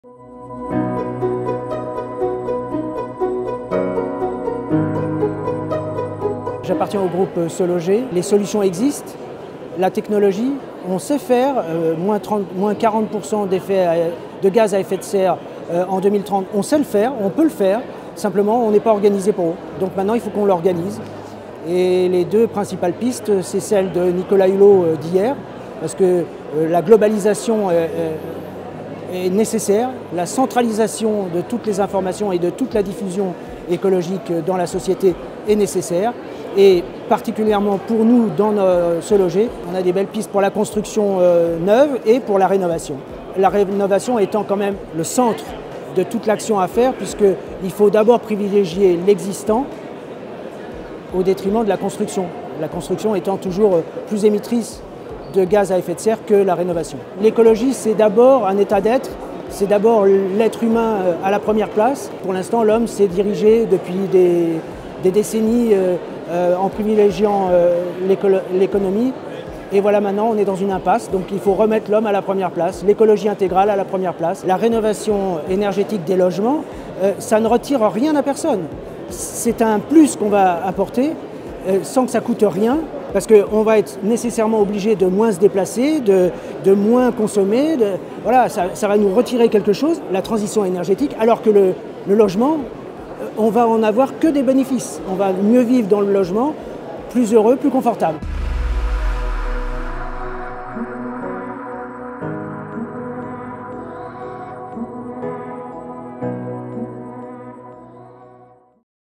Hugues Olivier - Session #3 convention citoyenne pour le climat
Hugues Olivier, membre de la convention citoyenne pour le climat, revient sur cette 3e session et nous parle d'écologie, de rénovation et des propositions qui commencent à émerger de son groupe "se loger".